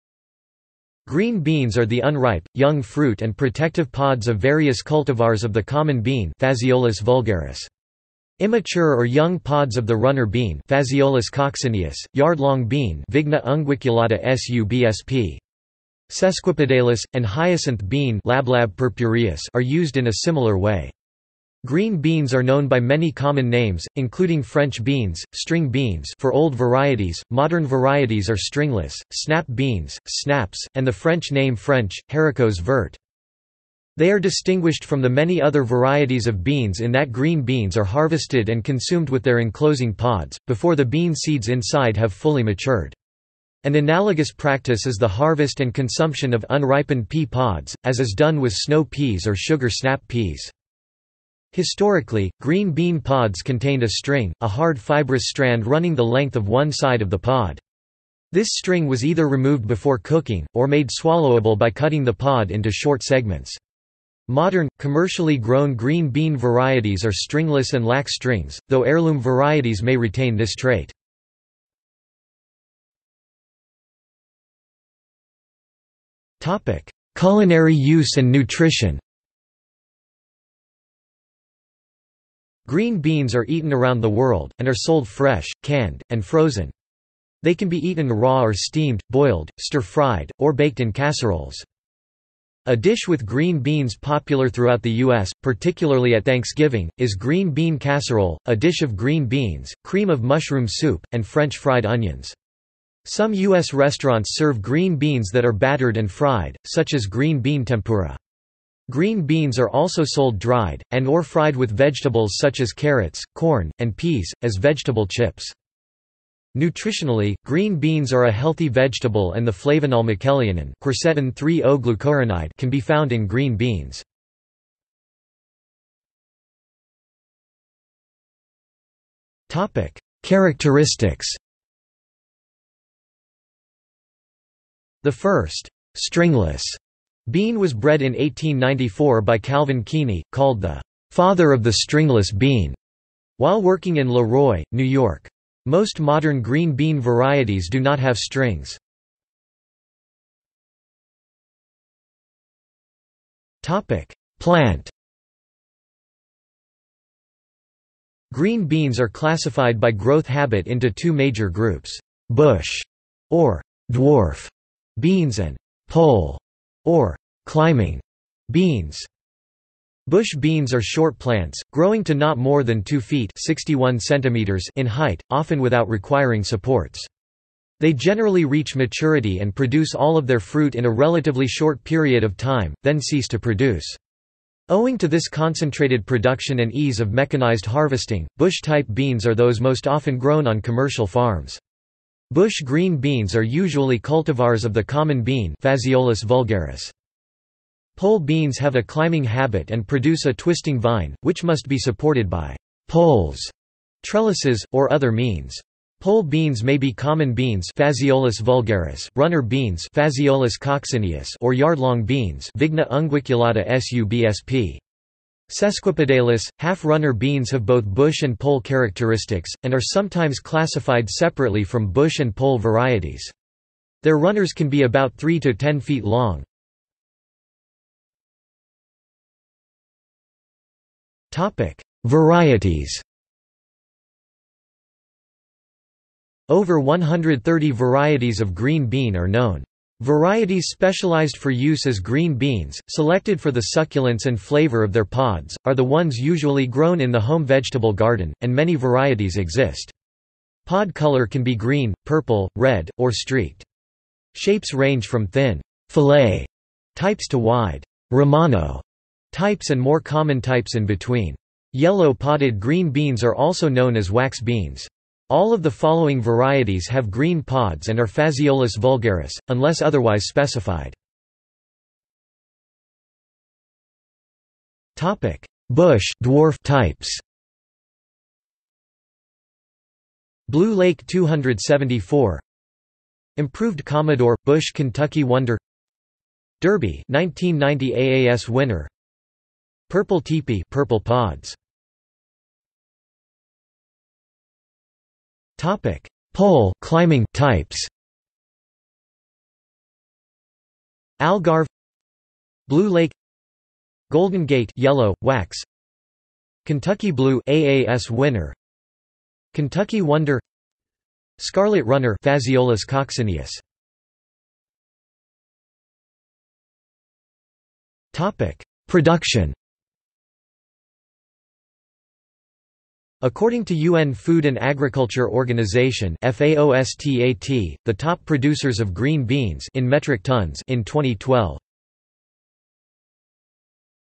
String bean | Wikipedia audio article
This is an audio version of the Wikipedia Article:
String bean

00:01:12 1 Culinary use and nutrition
00:02:05 2 Characteristics
00:02:30 2.1 Plant
00:03:48 2.2 Varieties
00:04:31 2.2.1 Bush (dwarf) types
00:04:52 2.2.2 Pole (climbing) types
00:05:14 3 Production

- Socrates

SUMMARY
Green beans are the unripe, young fruit and protective pods of various cultivars of the common bean (Phaseolus vulgaris). Immature or young pods of the runner bean (Phaseolus coccineus), yardlong bean (Vigna unguiculata subsp. sesquipedalis), and hyacinth bean (Lablab purpureus) are used in a similar way. Green beans are known by many common names, including French beans, string beans (for old varieties; modern varieties are stringless), snap beans, snaps, and the French name French: haricots vert.
They are distinguished from the many other varieties of beans in that green beans are harvested and consumed with their enclosing pods, before the bean seeds inside have fully matured. An analogous practice is the harvest and consumption of unripened pea pods, as is done with snow peas or sugar snap peas.
Historically, green bean pods contained a string, a hard fibrous strand running the length of one side of the pod. This string was either removed before cooking, or made swallowable by cutting the pod into short segments.  Modern, commercially grown green bean varieties are stringless and lack strings, though heirloom varieties may retain this trait.